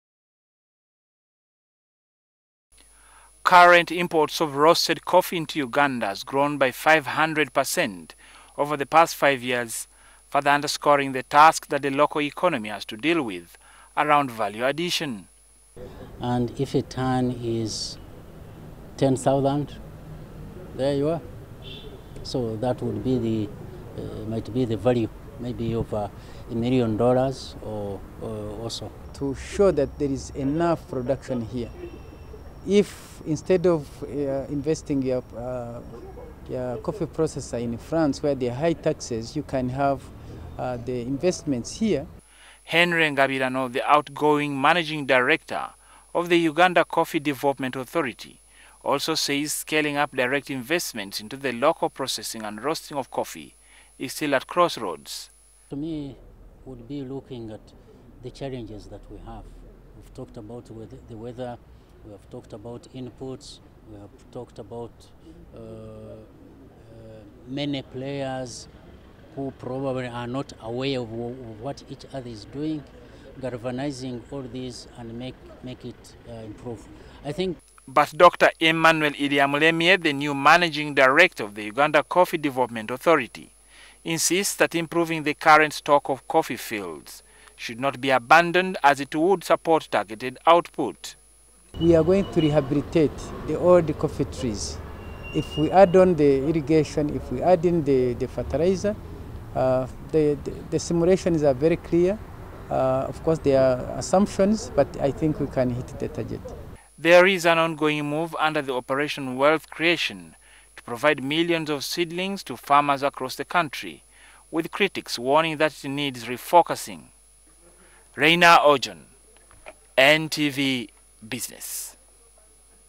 Coffee authority boss Ngabirano quits after 15 years
After 15 years, the man who has been at the helm of the Uganda Coffee Development Authority has bowed out having overseen most of the years of the coffee sector liberalization. Henry Ngabirano will leaves behind the task for his predecessor to grow the exports from the current 400 million dollar worth of export revenues.